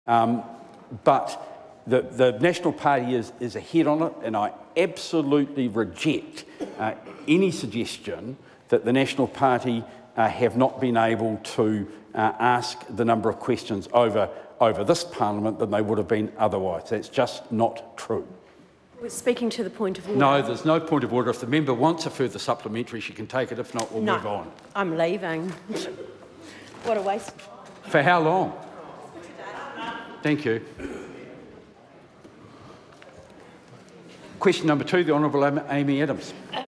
'Nah, I'm leaving, what a waste of time'
Exasperated Paula Bennett walks out of parliament after disagreement with Speaker Trevor Mallard.